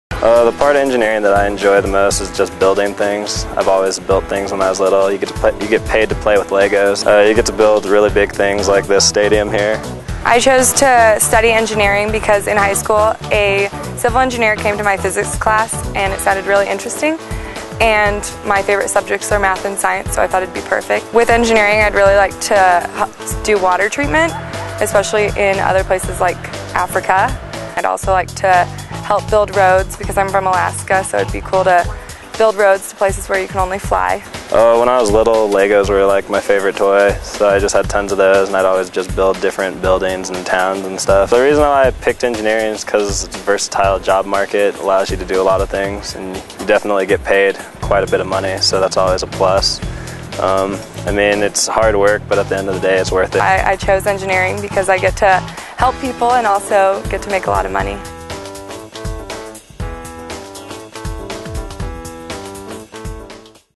Why Choose Civil Engineering?
Civil engineering students at the University of Colorado Boulder discuss their choice of major.